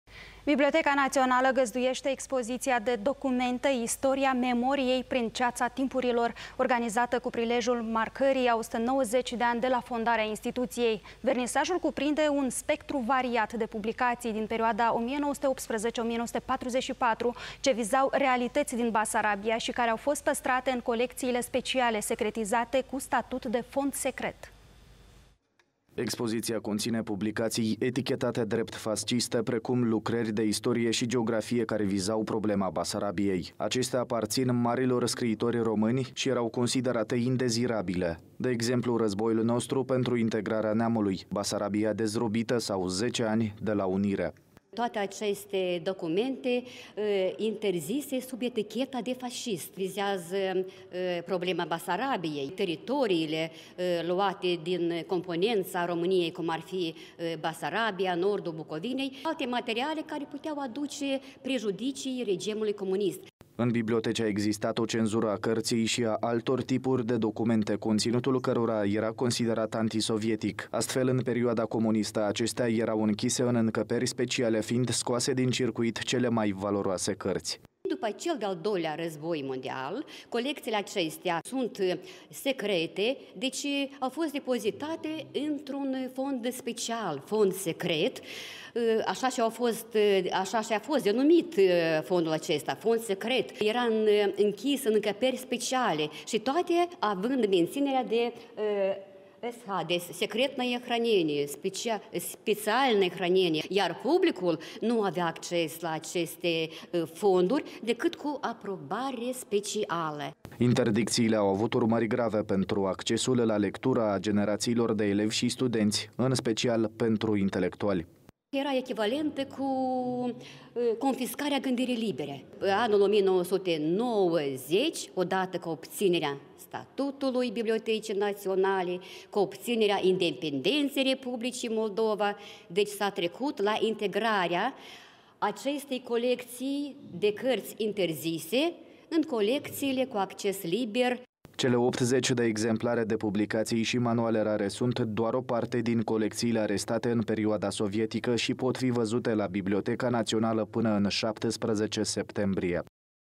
Expoziţie de documente „Istoria memoriei prin ceaţa timpurilor”, organizată la Biblioteca Naţională
Biblioteca Naţională găzduieşte expoziţia de documente „Istoria memoriei prin ceaţa timpurilor”, organizată cu prilejul marcării a 190 de ani de la fondarea instituţiei. Vernisajul cuprinde un spectru variat de publicaţii, din perioada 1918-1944, ce vizau realităţile din Basarabia şi care au fost păstrate în colecţiile speciale secretizate cu statut de „fond secret”.